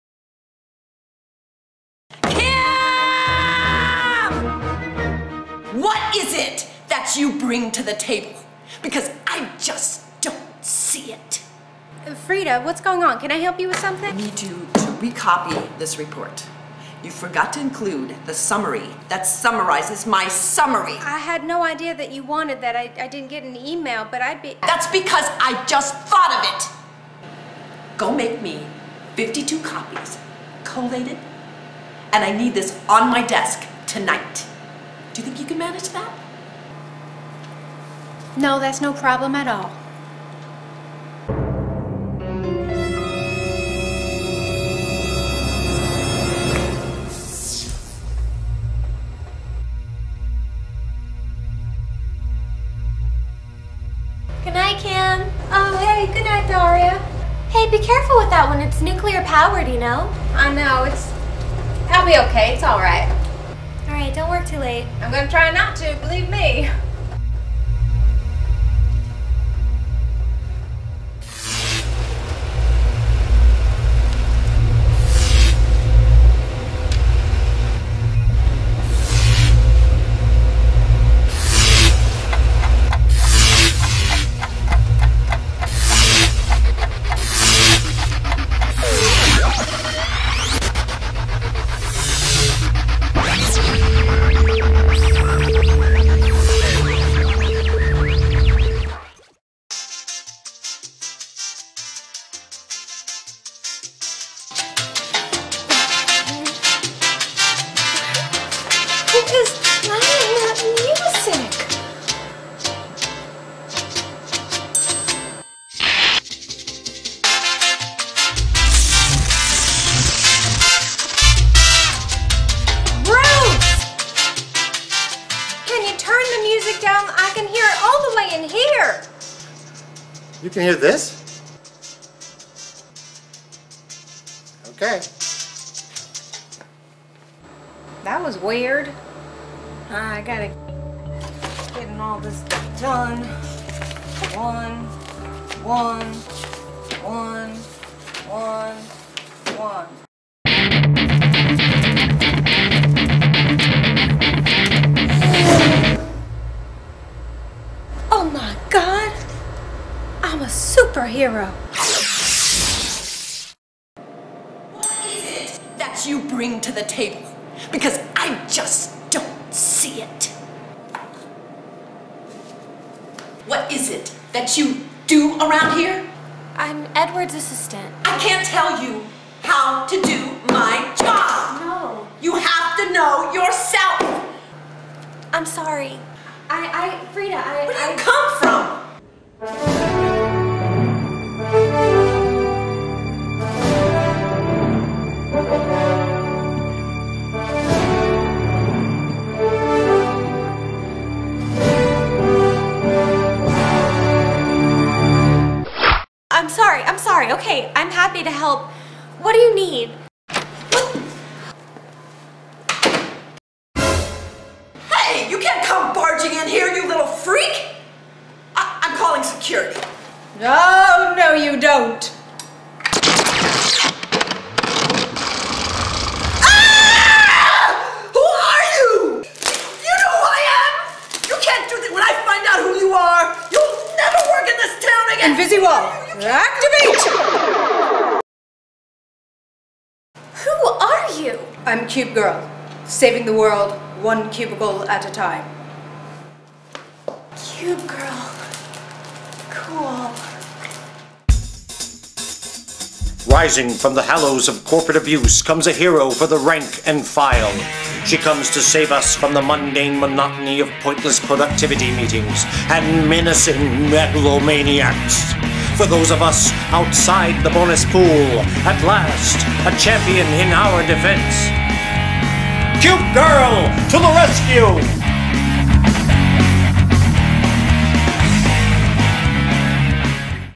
Cubegirl: The Origin
There's a new cape in town...her name is Cubegirl. Normal cubicle worker by day, and superhero when needed,  she's saving the world, one cubicle at a time.